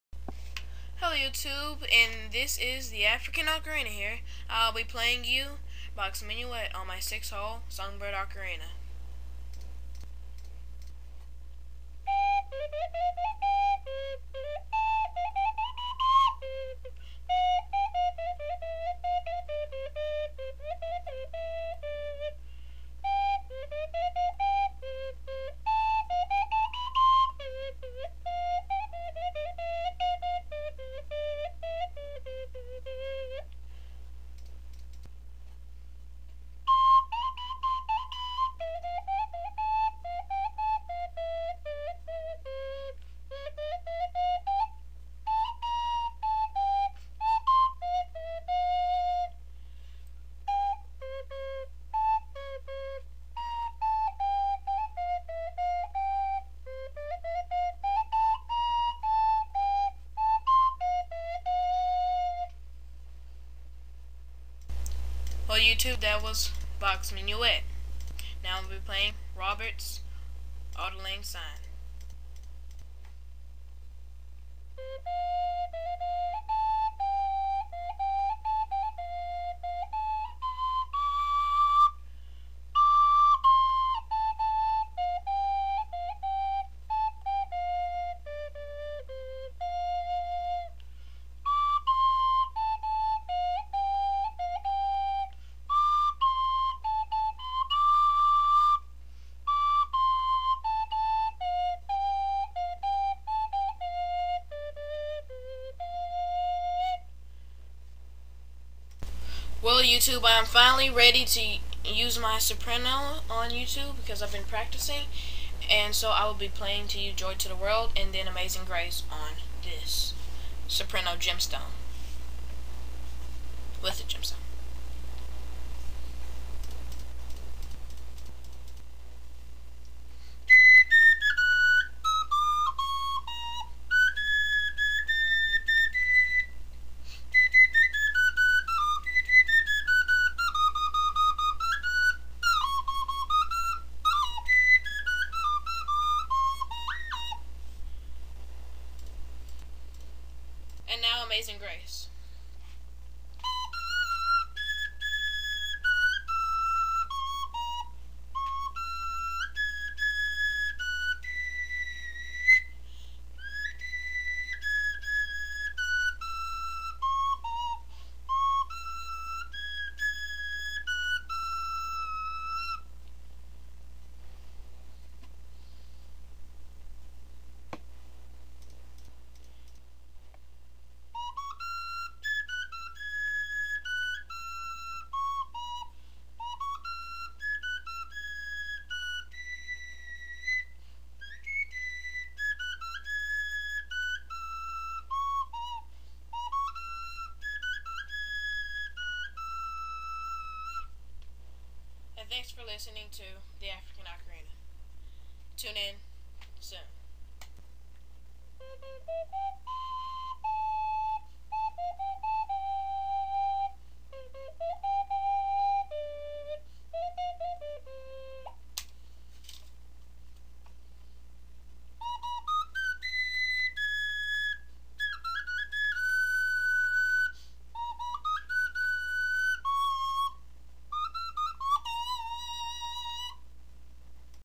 Classic Music with T.A.O.
A few classic with my two songbird ocarinas, (tenor and saprano). I hope you enjoy. *Pictures are Clips are of composers of music)